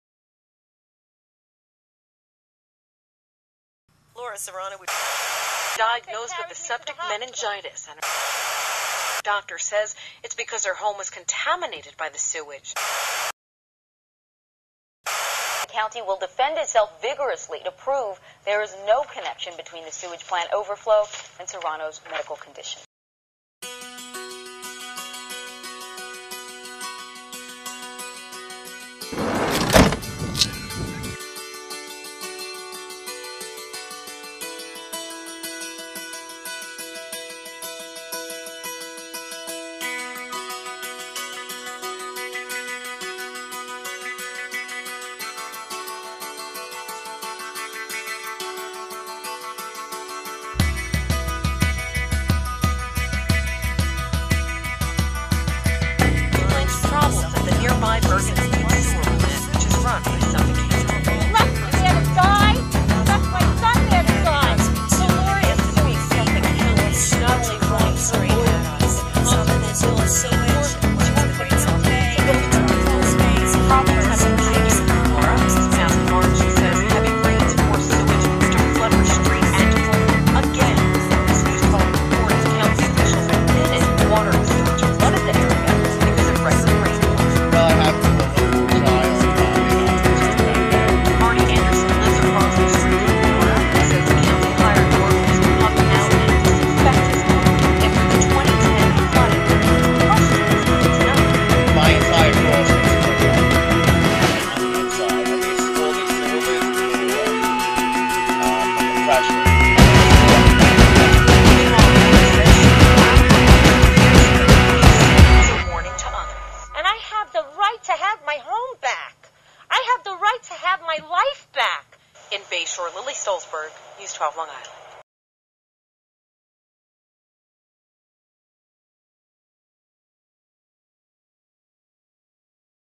Raw Sewage Documentary Trailer
This is a trailer for a documentary I am currently working on for Laura Serrano.